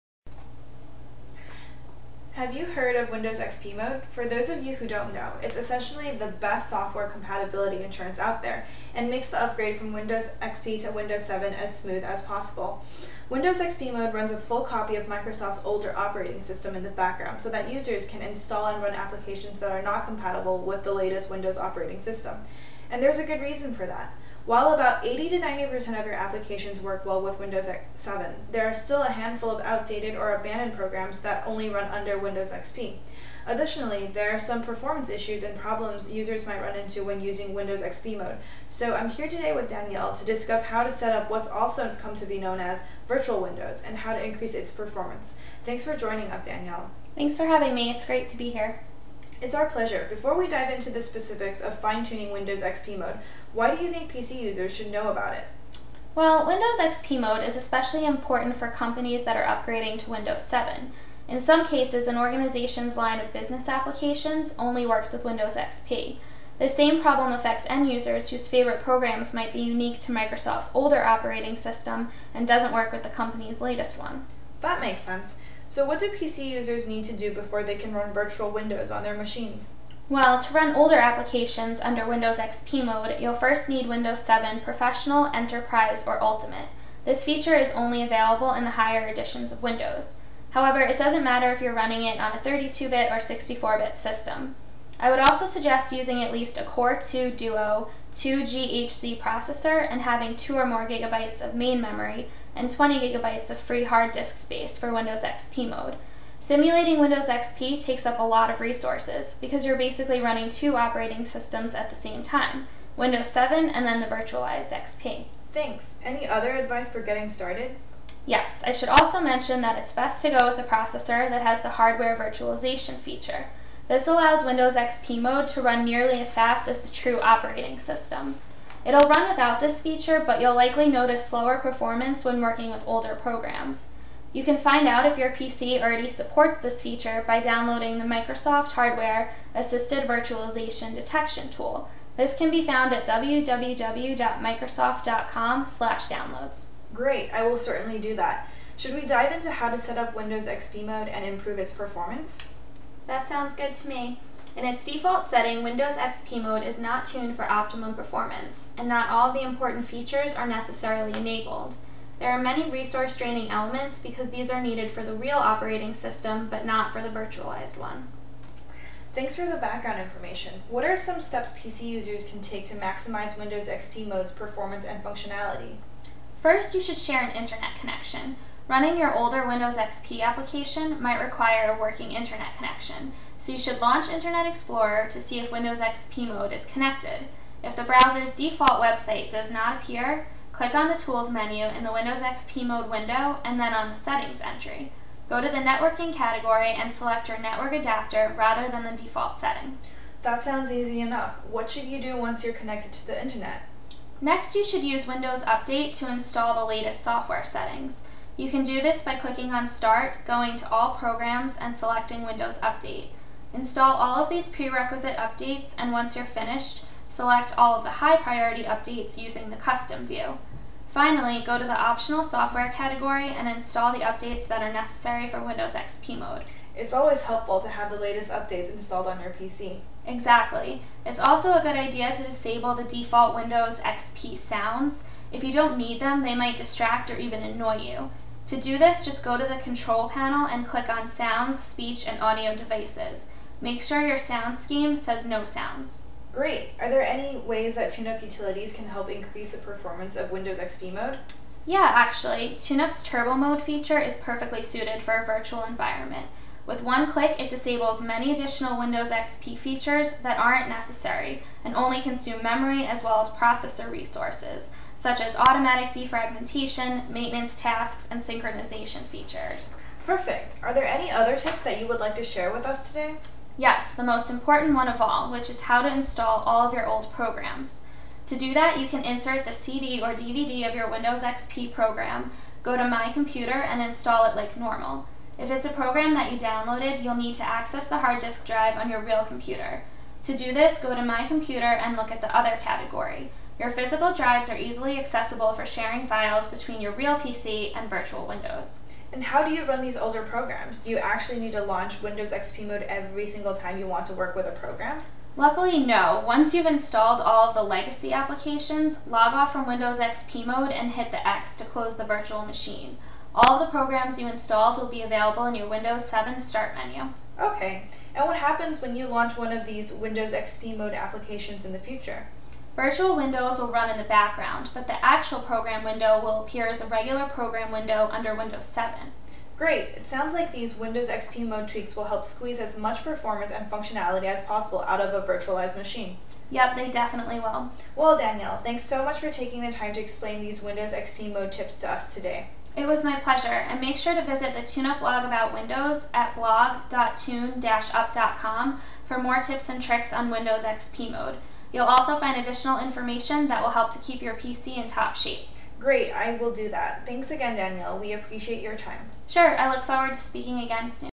Podcast: Maximizing Windows XP Mode's Performance
In this podcast, we'll explain how to set up Windows XP Mode, maximize its performance and tweak "Virtual Windows" to your liking. Tune in to make sure you're up to speed on all of the tips and tricks available for Windows XP Mode!